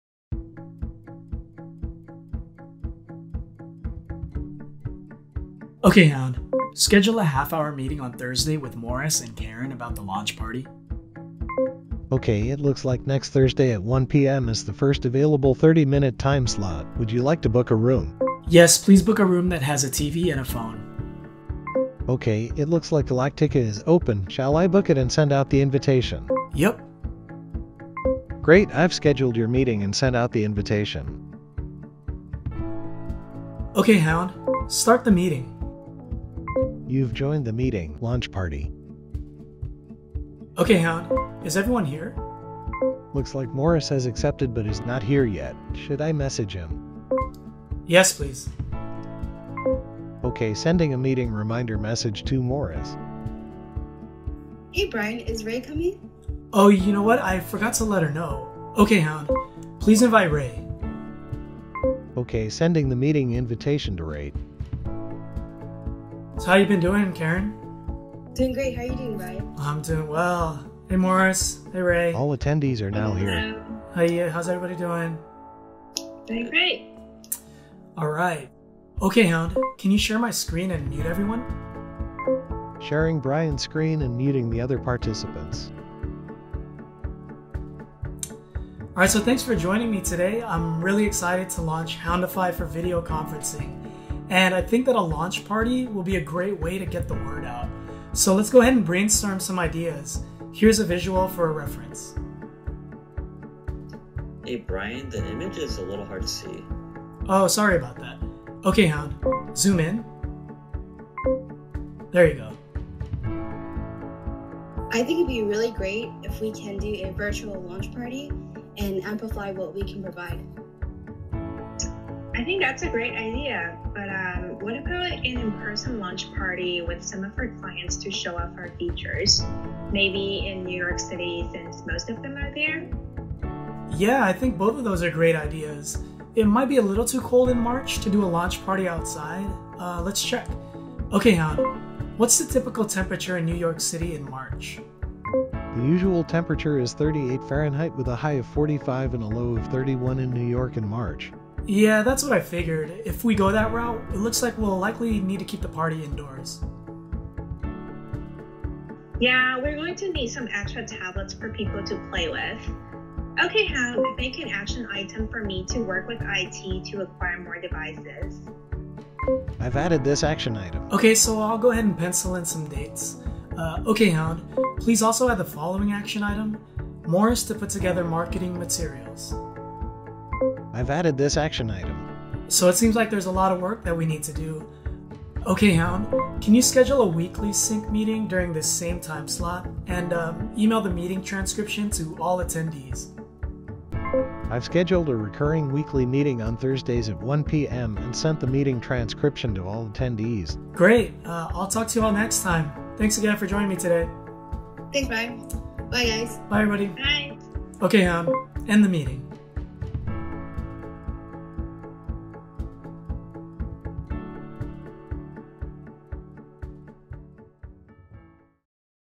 DEMO: AI-Powered Voice Assistant for Video Conferencing and Meetings
Houndify's advanced voice AI can transform video calls and meetings into more efficient, convenient, and hands-free experiences.

Our proprietary Speech-to-Meaning ® and Deep Meaning Understanding™ technologies can help you build a custom voice assistant that quickly and accurately understands simple and complex commands. Easily schedule calls, send out meeting invites, run smoother meetings, share documents, and create and assign action items all via natural voice queries.

For example you can say things like:
“Schedule an hour-long meeting with Mike and Sarah about the 2021 marketing plan”
“Share my screen and mute all other attendees”
“Add an action item for Julie to put together the social report for September”
“Please add Jason to the meeting”
“Schedule a follow-up meeting for next Tuesday”

By voice-enabling video calls, companies can streamline video calls, and save time and effort so your teams can focus on what’s important: connecting meaningfully and exchanging ideas that move your business forward.